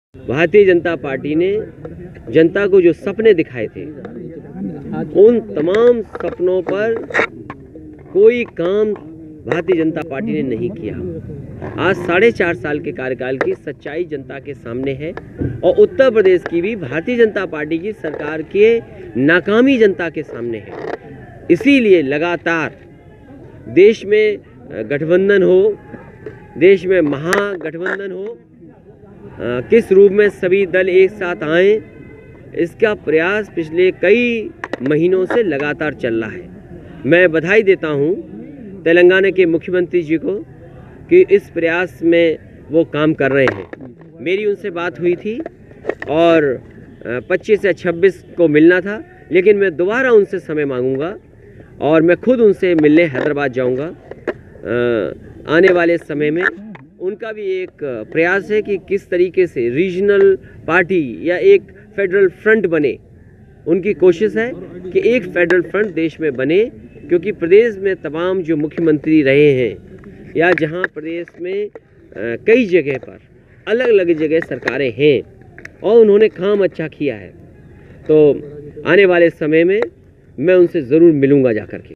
तेलंगाना के सीएम केसीआर से मिलने हैदराबाद जाऊंगा: अखिलेश यादव
लखनऊ: यूपी के पूर्व मुख्यमंत्री और सपा अध्यक्ष अखिलेश यादव ने कहा कि नए साल में तेलंगाना के सीएम के. चंद्रशेखर राव से मिलने हैदराबाद जाएंगे. अखिलेश यादव ने फेडरल फ्रंट का समर्थन करते हुए कहा कि अब हम चाहते हैं कि देश में एक फेडरल फ्रंट बने. इसके लिए मैं तेलंगाना के मुख्यमंत्री से मुलाकात करूंगा.